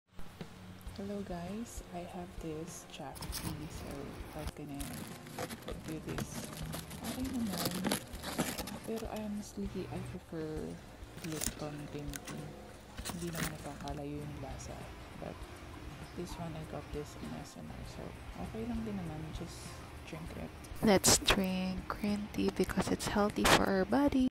JAF GREEN TEA unboxing PHP 129 for 20 tea bags - healthy living greentea tealover jaftea❤️
Green tea good for our body :)

I bought this online in SNR,
20 teabags for PHP 129 only!